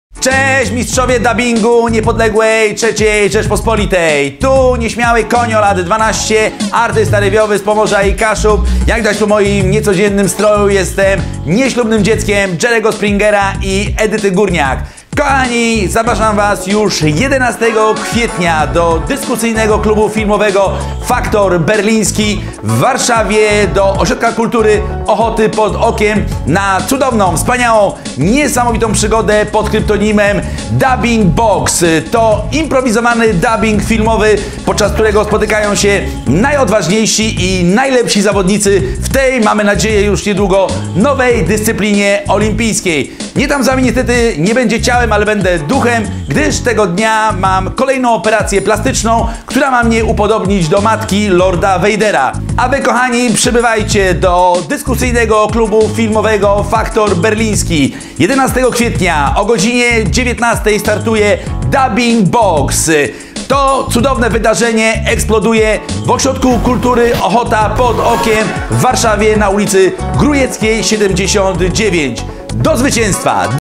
Konjo zapowiada Dubbing Box
Paweł Konjo Konnak zapowiada
Festiwal Dubbingu  "Dubbing Box"
11 kwietnia Warszawa
Ośrodek Kultury Ochota "Pod Okiem"
DKF "Faktor Berliński"
ulica Grójecka 79